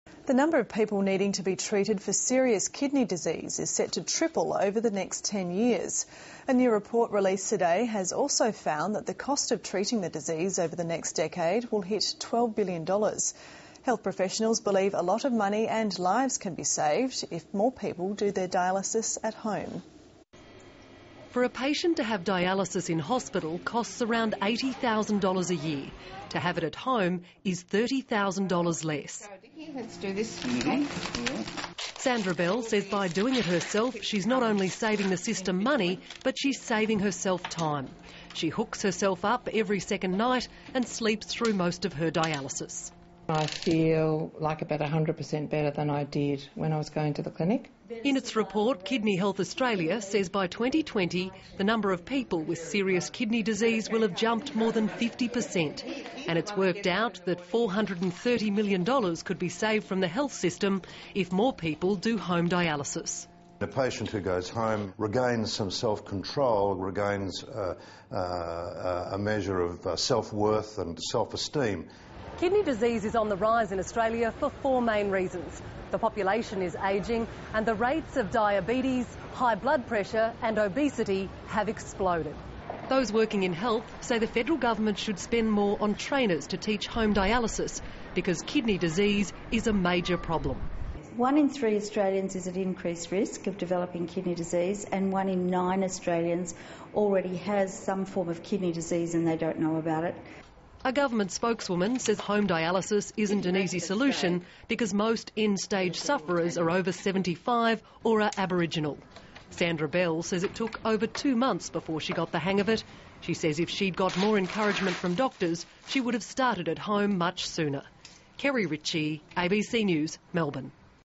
Study recommends kidney dialysis patients stay home
A report has been released in Melbourne revealing the cost of treating people with kidney dialysis is set to increase dramatically and that it may be better to be treated at home.